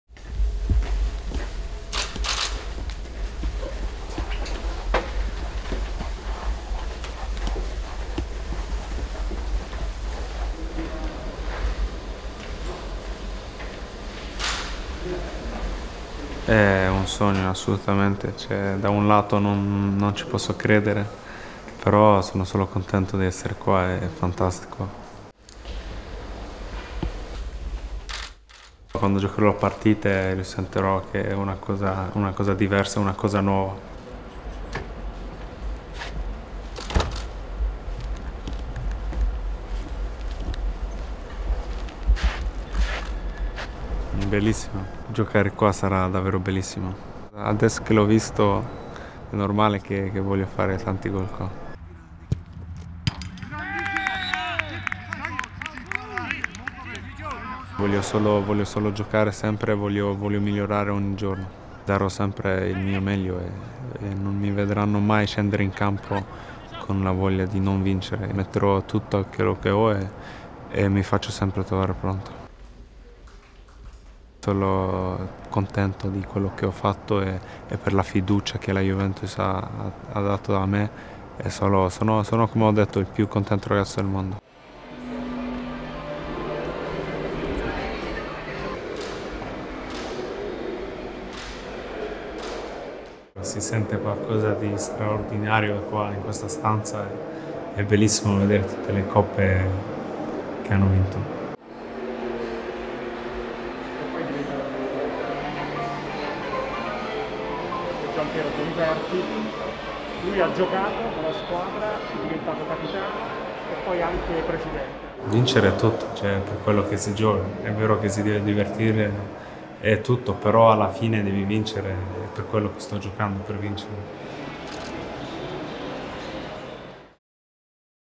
✨ WELCOME DEJAN! | Kulusevski Tours Juventus Museum & Allianz Stadium
Today, last season's Serie A Best Young Player and Swedish International Dejan Kulusevski was officially presented as a Juventus player at the Allianz Stadium!